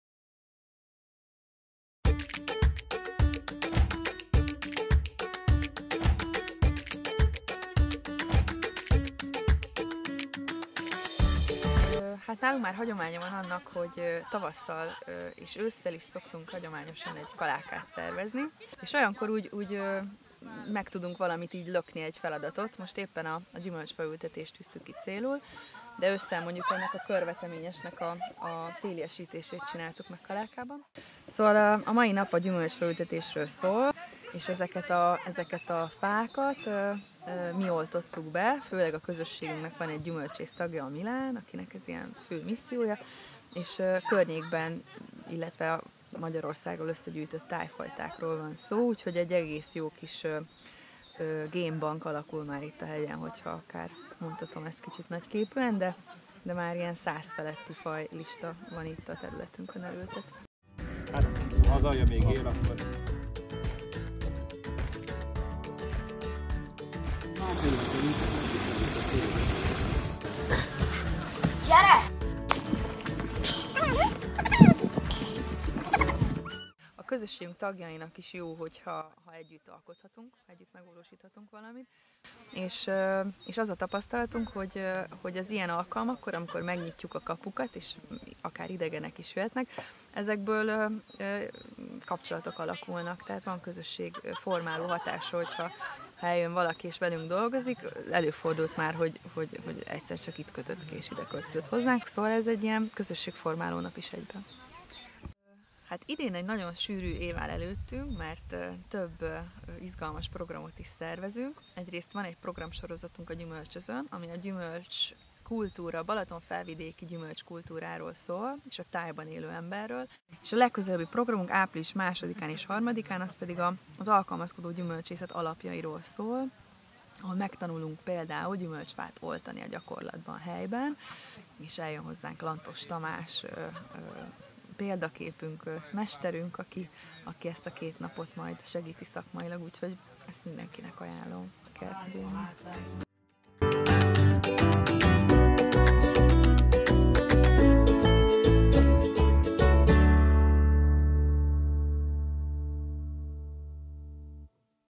Tavaszi kaláka a Hideghegyen
Tavaszi faültetés a Hideghegyi Menedékben, Regivel, Balázzsal, Endrével és a többiekkel...